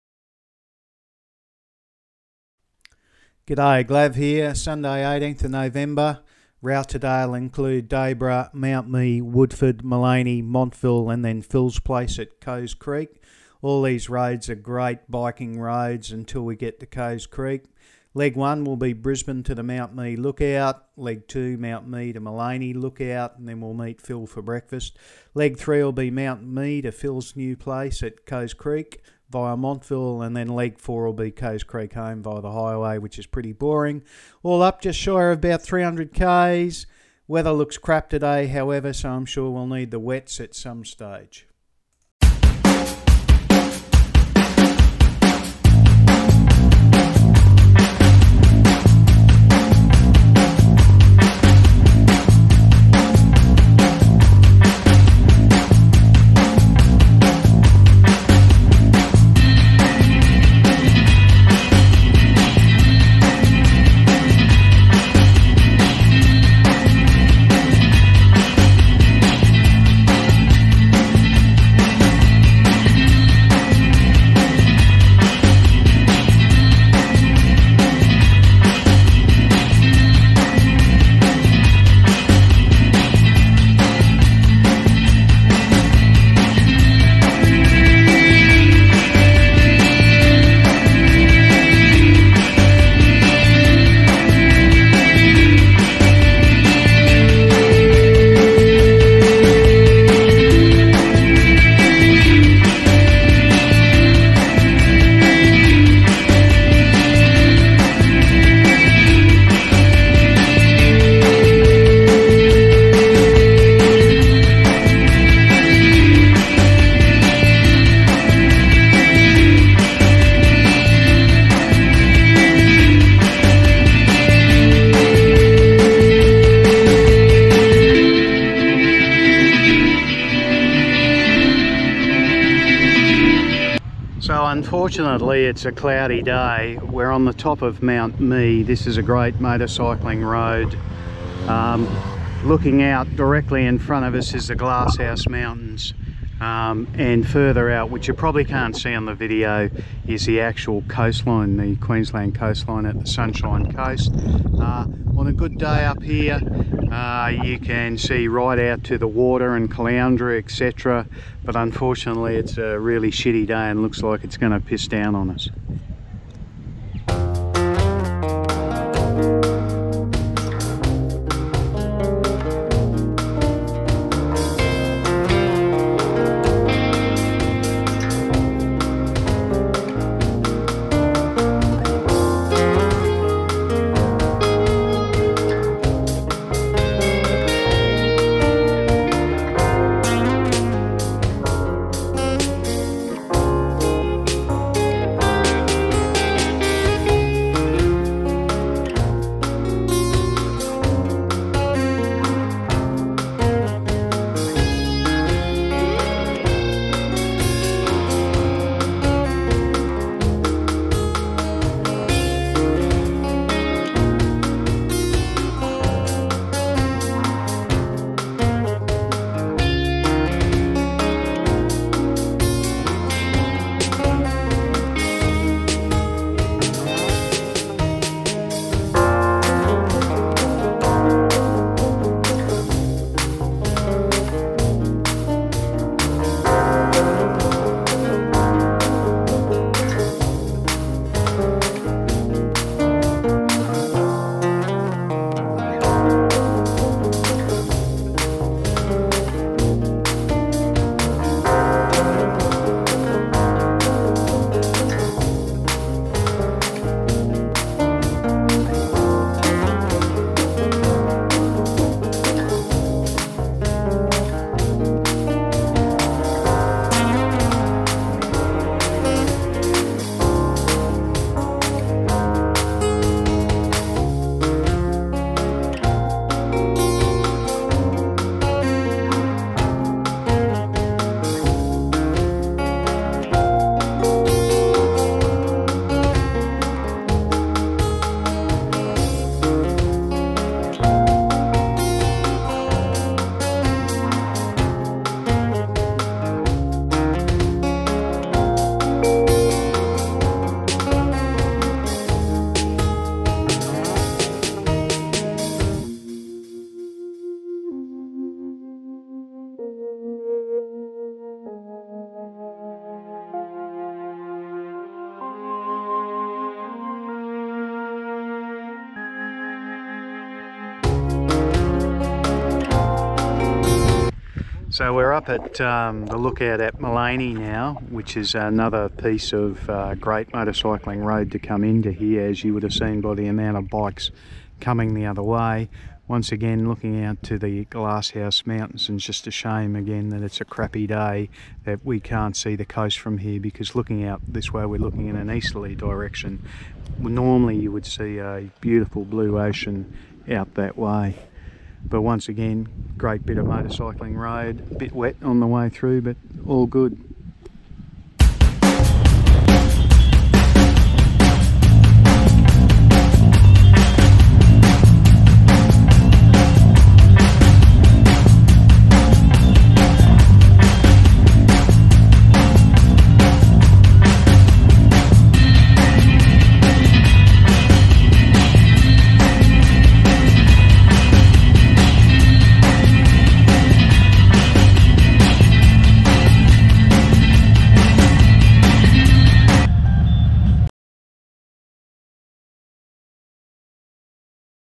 Glav's World Ride to Maleny (QLD)
A ride from Brisbane to Dayboro, Mt Mee, Woodford, Maleny, Montville and Coes Crrek. Approximately 300 Kms (round trip). This is one the great Day Rides in South East Queensland. It has everything for the Motorcyclist with breathtaking scenery.